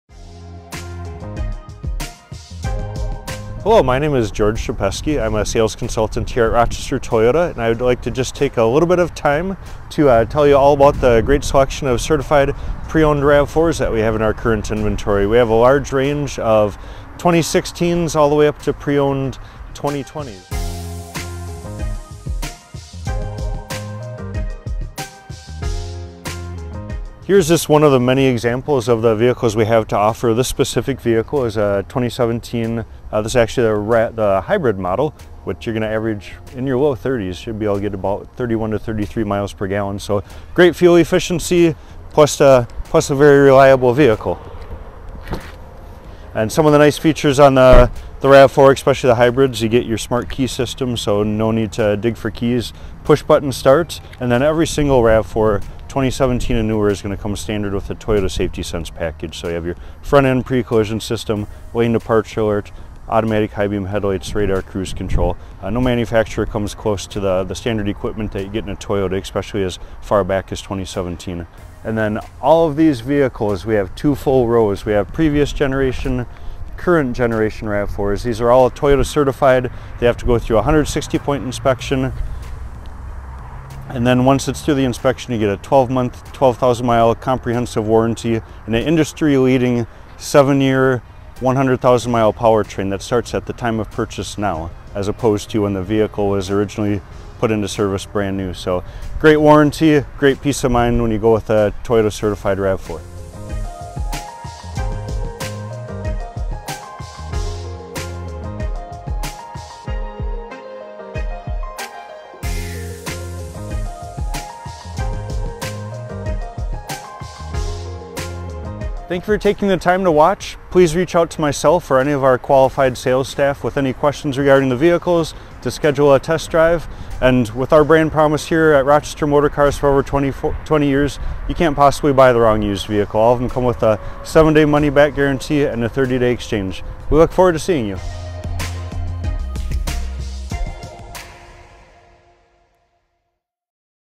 Rochester Toyota - Used CPO Rav4s Available Now!
Our Sales Consultant, George, shares information about Toyota's wide variety of Certified Pre-Owned Rav4s we have available RIGHT NOW! and stop by Rochester Toyota to test drive a Rav4 today.